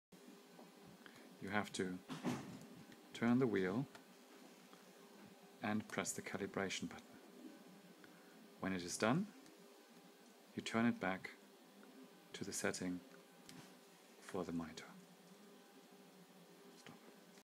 Color Management 1: Color Munki self calibration
This video shows how to set your Color Munki into the self calibration mode.